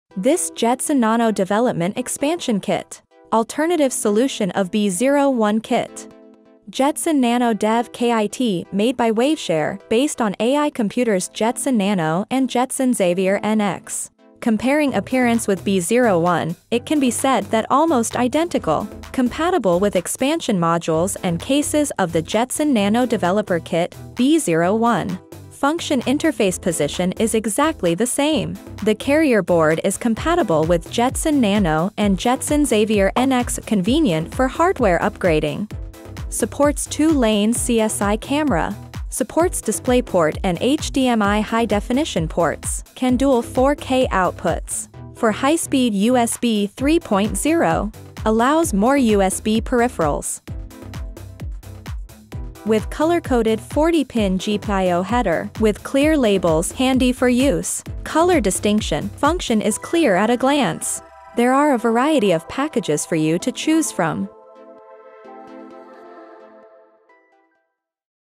Jetson Nano Development/Expansion Kit, Alternative Solution Of B01 Kit, Optional Nano Core Module
JETSON NANO DEV KIT made by Waveshare, based on AI computers Jetson Nano and Jetson Xavier NX, providing almost the same IOs, size and thickness as the Jetson Nano Developer Kit (B01), more convenient for upgrading the core module.